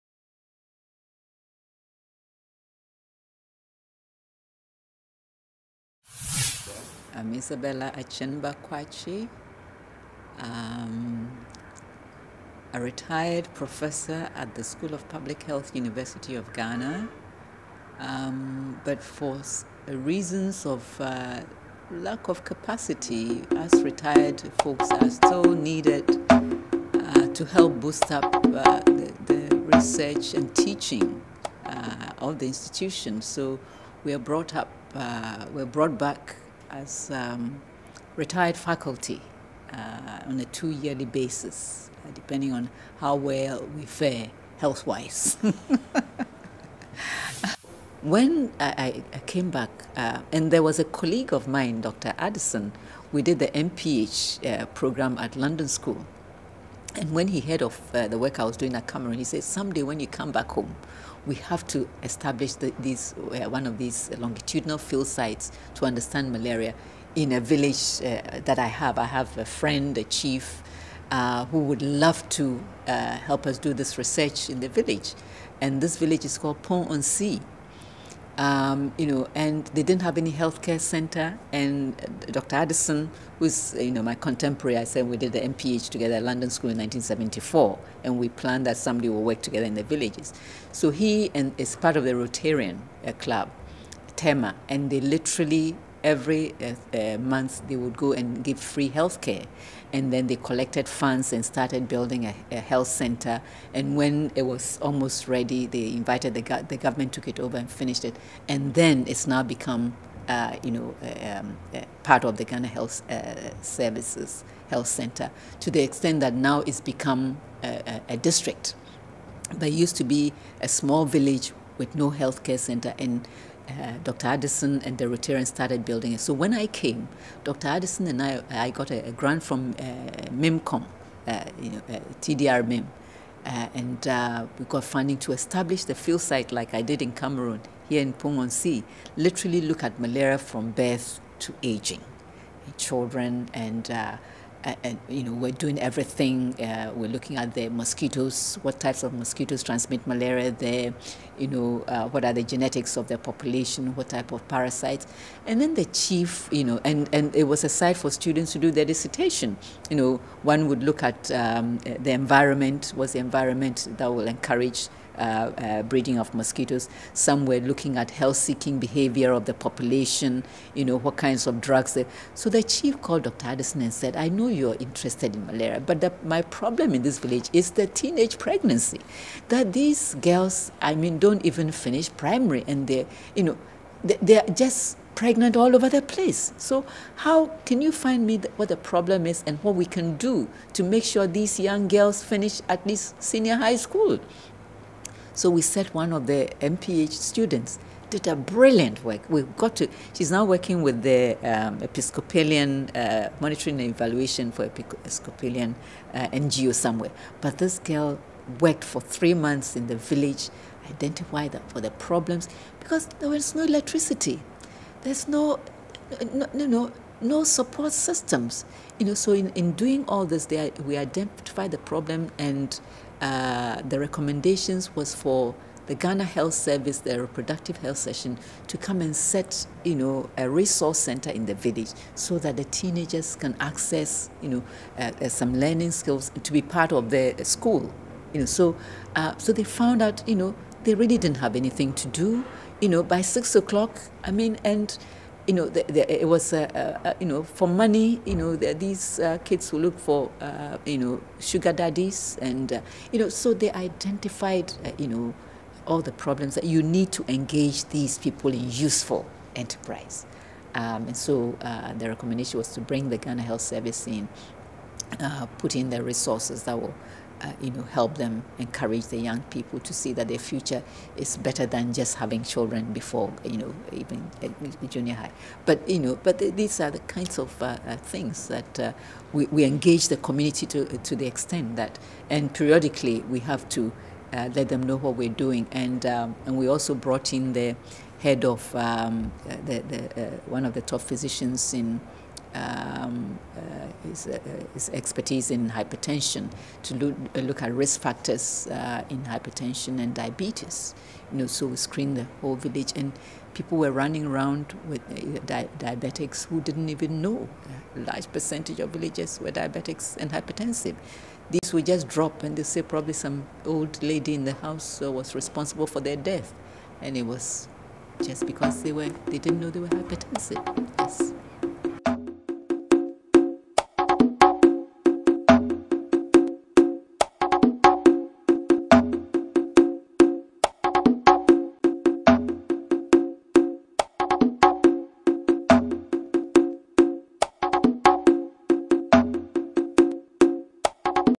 Prof. Quakyi Isabella - University of Ghana,
Isabella Quakyi of University of Ghana, Accra Legon with expertise in Human Biology, Immunology, Parasitology.
Professor of Immunology and Parasitology.